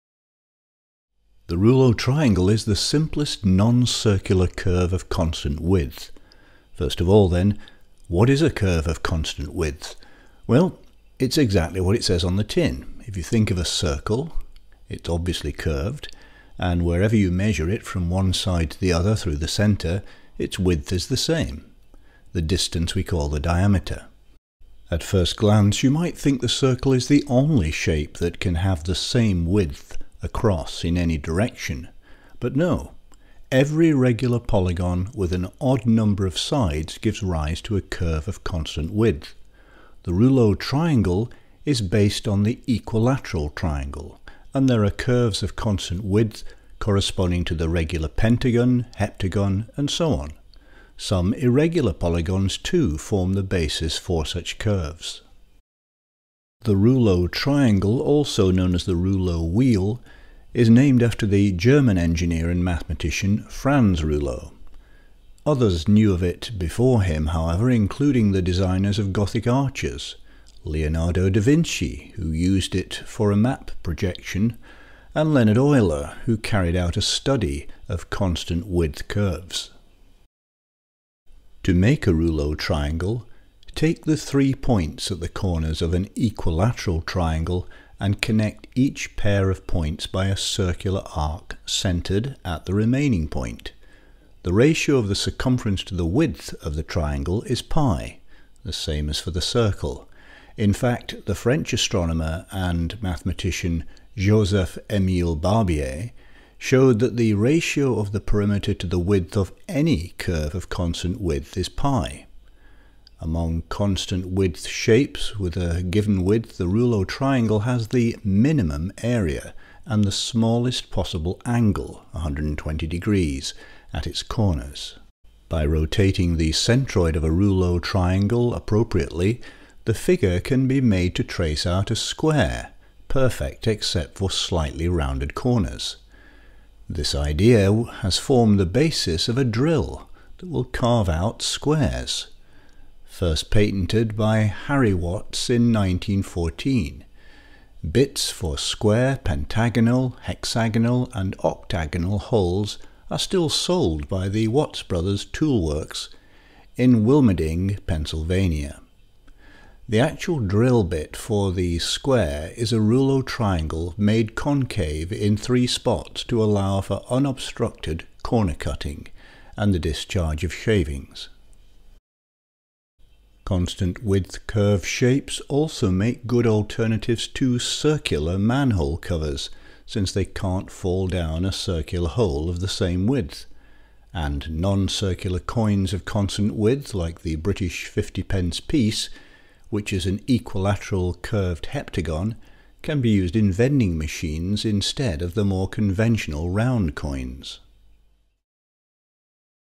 The remarkable Reuleaux triangle
The Reuleaux triangle is the simplest noncircular curve of constant width.